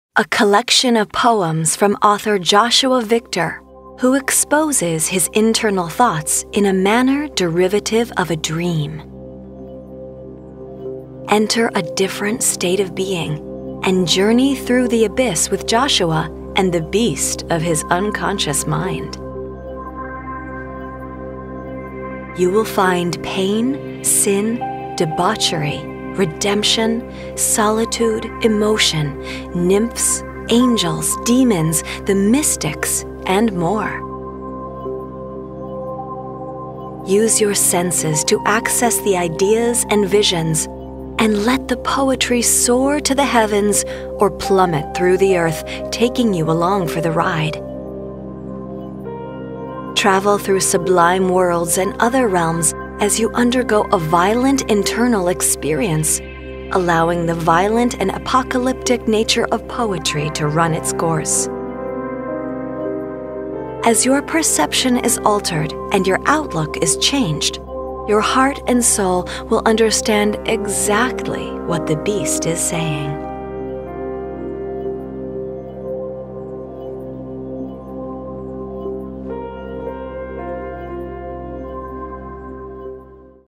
The Recluse and Other Mystics by Joshua Victor
Journey through these abysses, reader. Make yourself available to the paladin. Walk to the threshold and open the door. Here you’ll find pain, sin, redemption, debauchery, vulnerability, subtlety, solitude, addiction, and the mystics—of course, everything pliable to the human heart. Use your senses to understand the ideas of the beast. These are also visions of my phantasm, but very much so accessible real realms to the senses. Real poetry either soars to the heavens or plummets through the earth. Poetry is violent and apocalyptic. I know none other as sure.